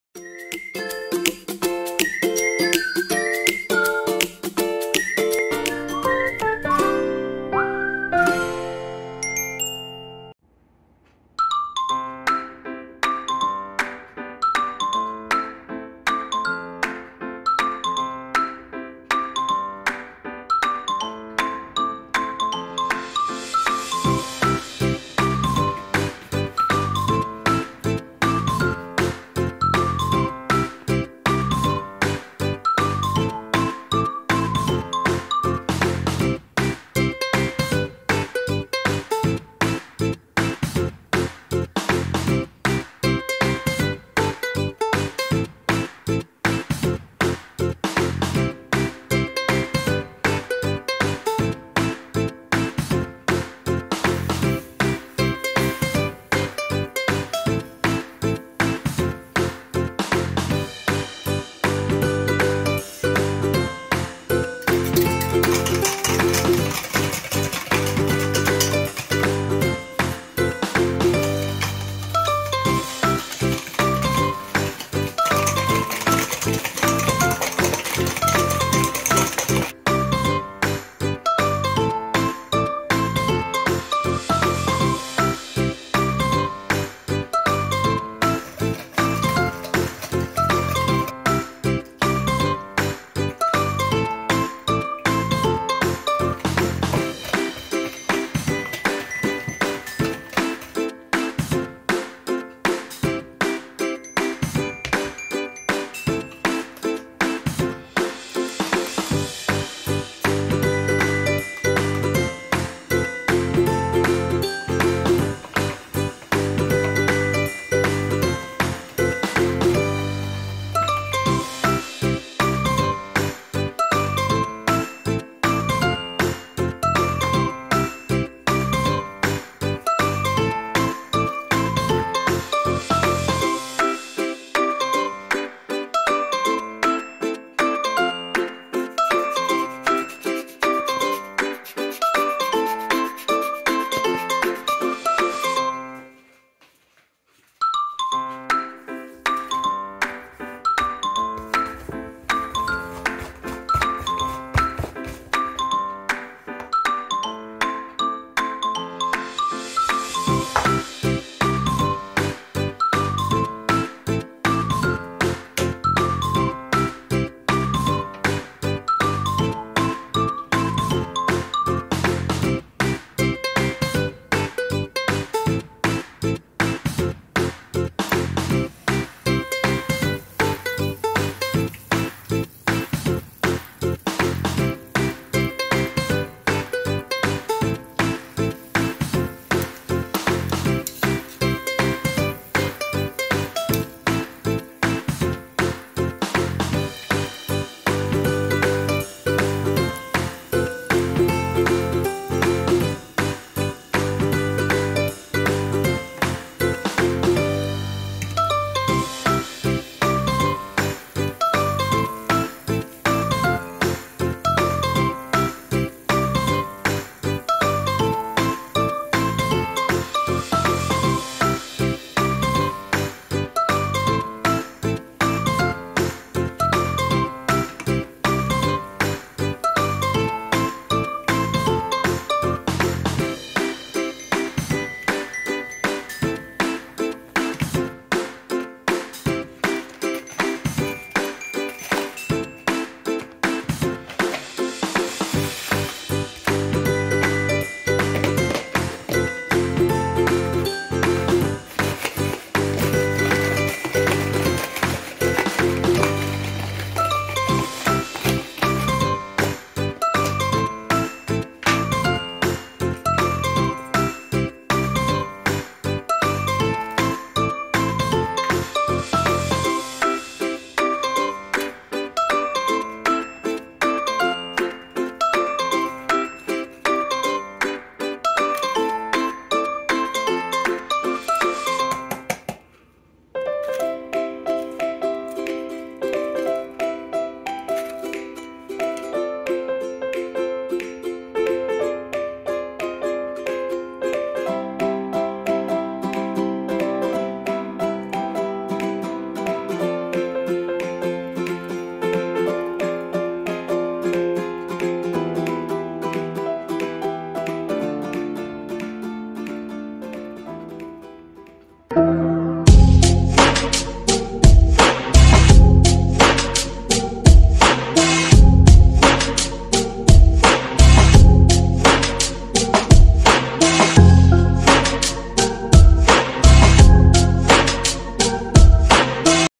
Jednoduché rychlé koblížky-recept
Složení:

250-300 g mouky
250 g jogurtu
150 ml mléka
1 vejce
1 lžička citronové kůry
70 g cukru (5 lžíce)
10 g prášku do pečiva (1 polévková lžíce)
rostlinný olej
cukr na ozdobu